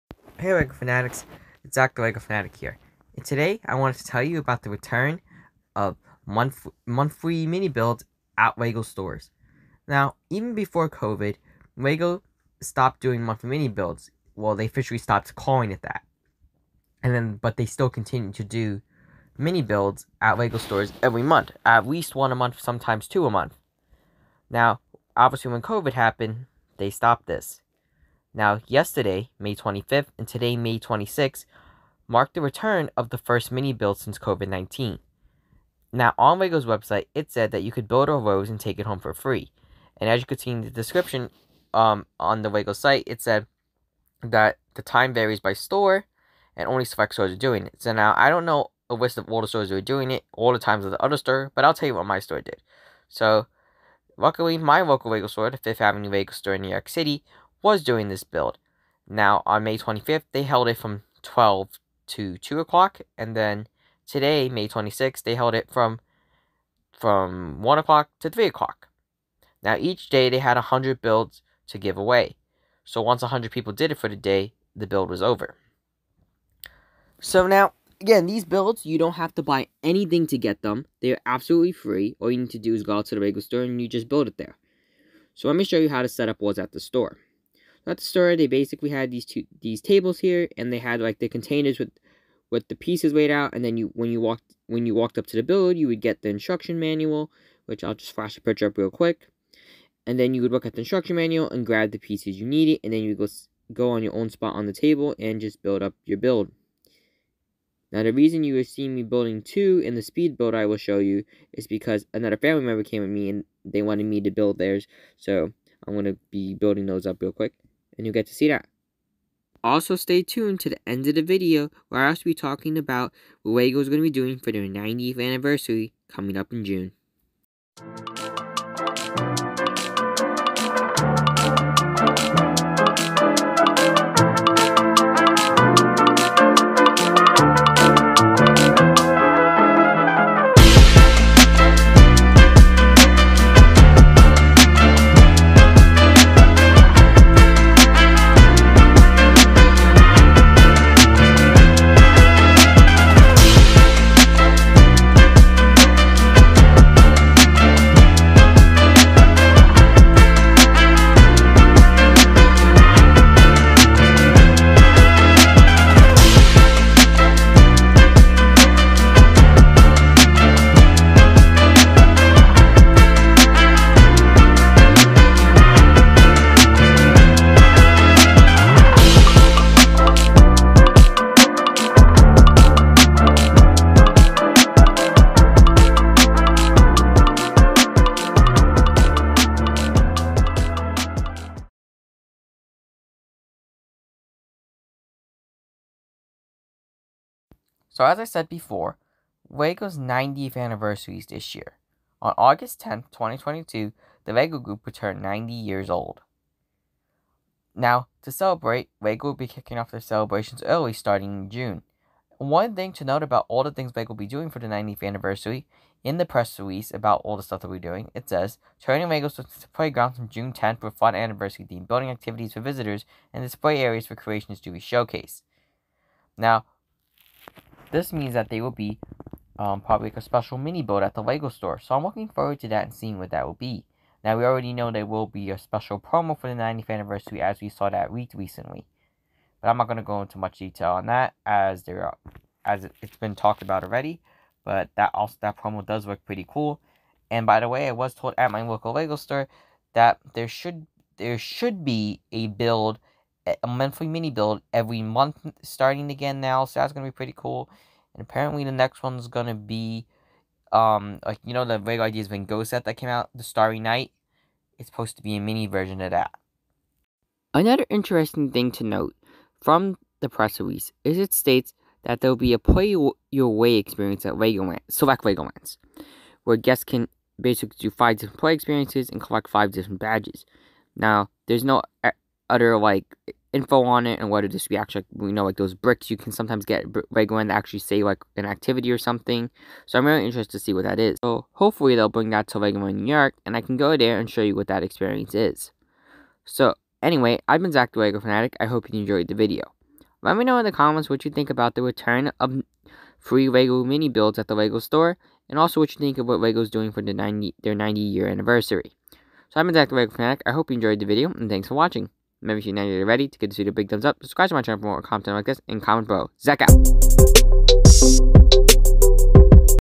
Return of Free Lego Make and Take Builds at The Lego Store! Special Lego 90th Anniversary Events!
Hey Lego fanatics, it's Zach The Lego Fanatic and in this video I will be showing you the first free make and take build at the Lego Store since the pandemic. If you recall, before covid Lego stores used to have at least one free make and take build every month. When Covid happened, this was suspended, but it looks like they are starting to make a comeback. On May 25th and 26th, 2022, select Lego stores held a make and take build of a Rose. There's info about the build and others to follow in the future, I also talk about the upcoming Lego 90th Anniversary events!

Hope you continue to stay safe and healthy.

Zach Out!